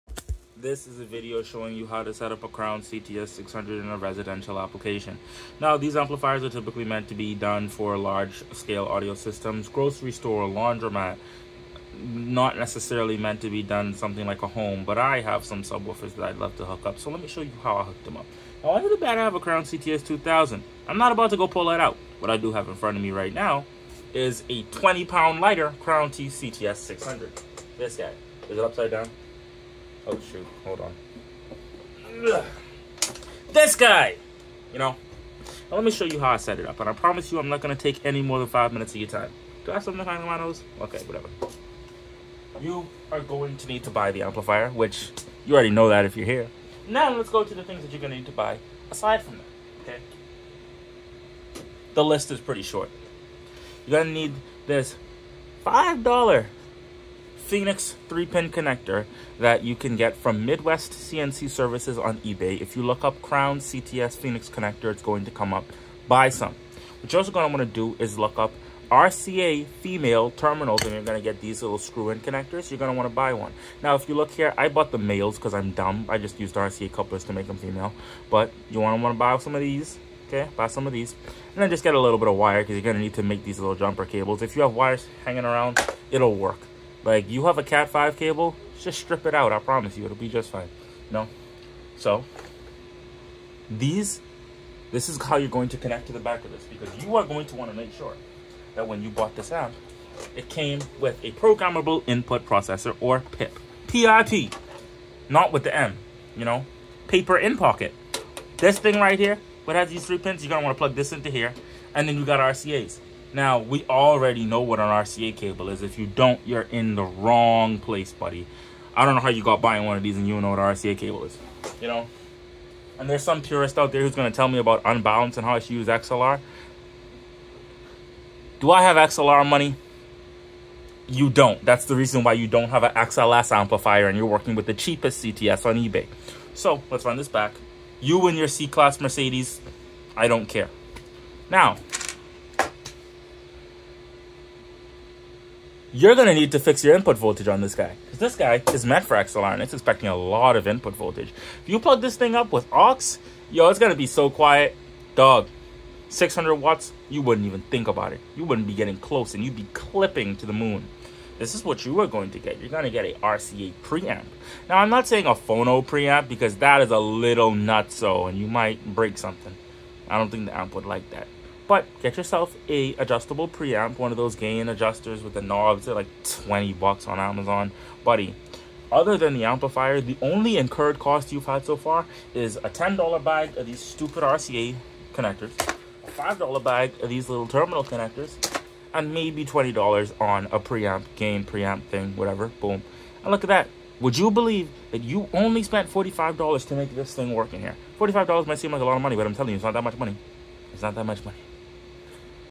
How to set up Crown CTS amplifier for a residential application
There are a million videos on the internet documenting the XLS series and there are about two in total even showing what the CTS looks like. I decided to create this video as someone who basically figured it all out to save other people the hassle. Also, if you're interested in the specific specs about these amps and some features that they have, Google Crown CTS and the manual should pop up on their website.

Corrections:
it's $35. I can't count.
Youre looking for a LINE LEVEL BOOSTER, not an RCA pre amp (although essentially similar)
I never mentioned the wiring diagram either, with the screws facing to the top, the leftmost is RCA positive (+). bridge the two left to make RCA negative. (-)